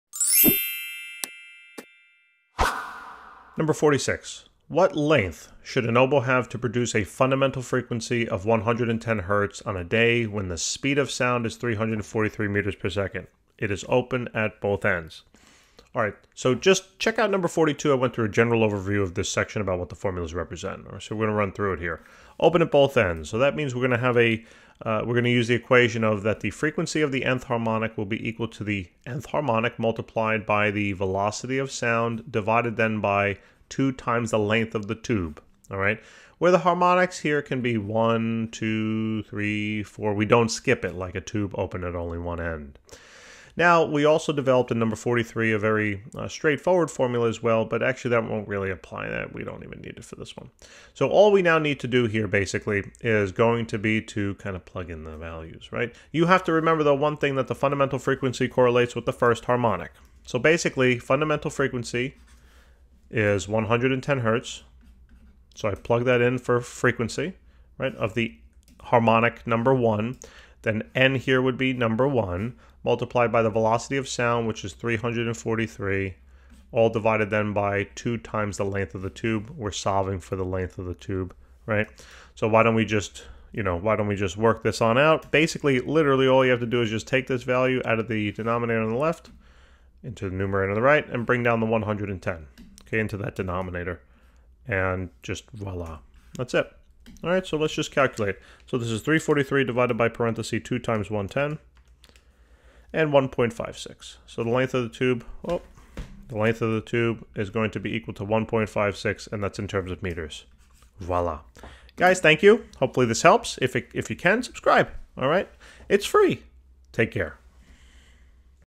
17.46 | What length should an oboe have to produce a fundamental frequency of 110 Hz on a day when
What length should an oboe have to produce a fundamental frequency of 110 Hz on a day when the speed of sound is 343 m/s? It is open at both ends.